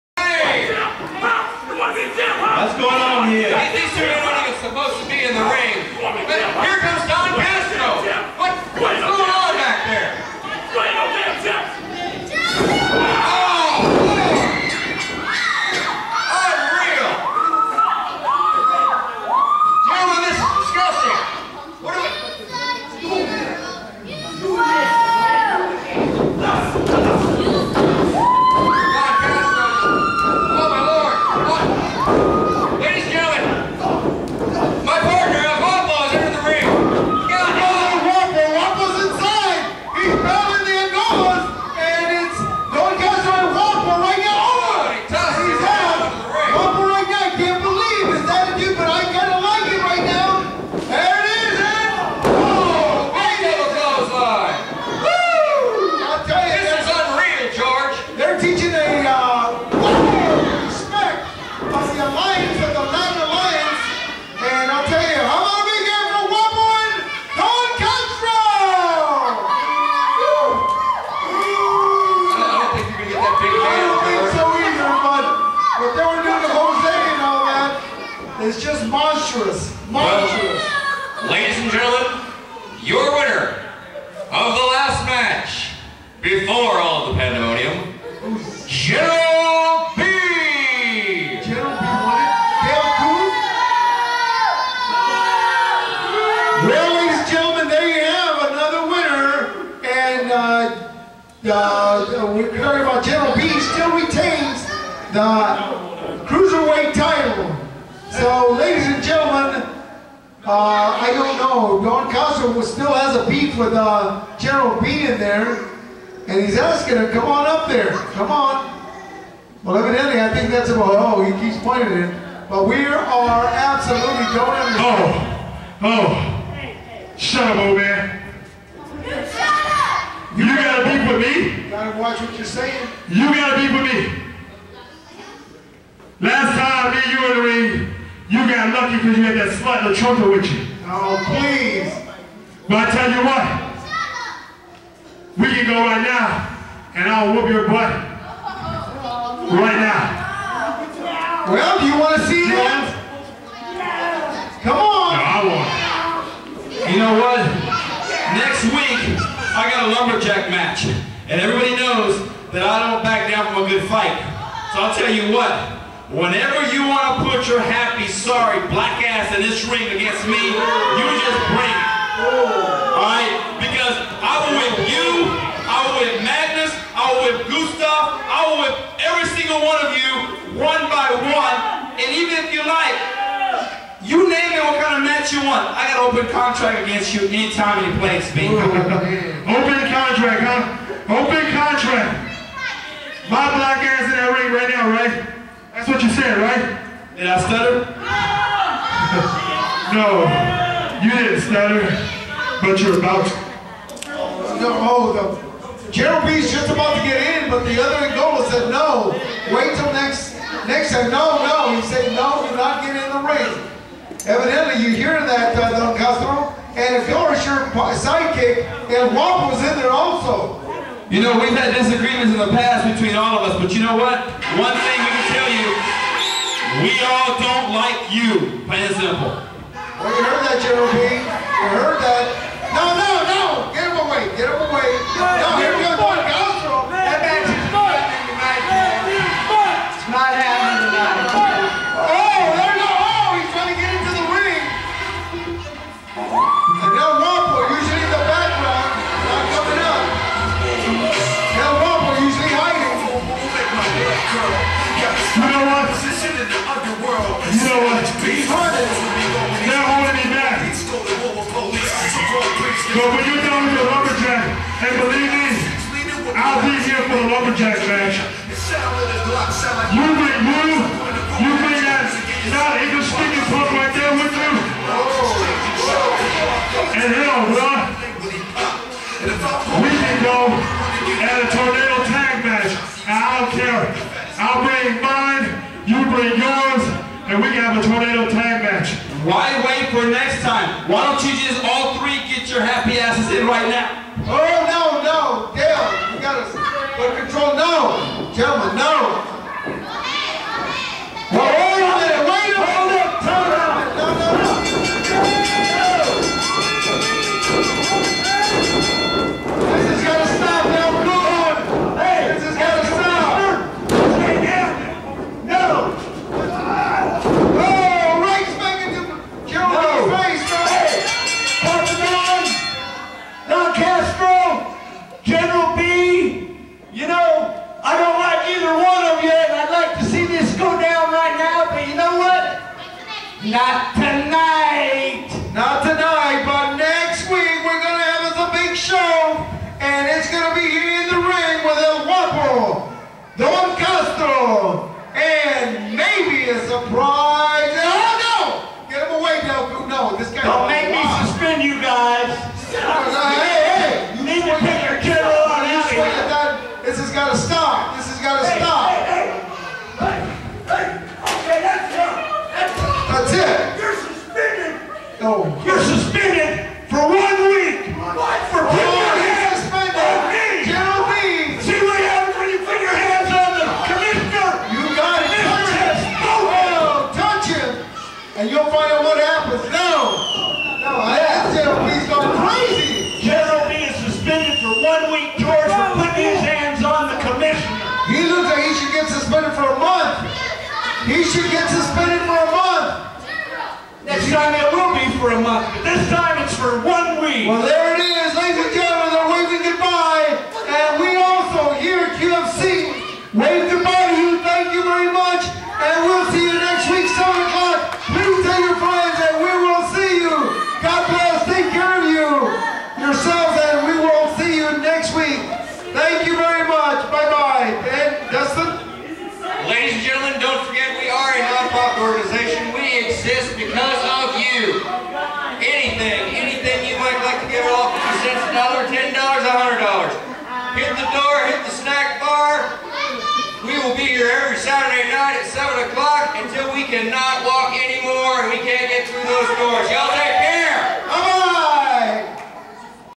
QFC SNX 3-8-8.8 JoJo Bravo vs The General (3of3)
Conclusion of JoJo Bravo and The General with quite a few run-ins.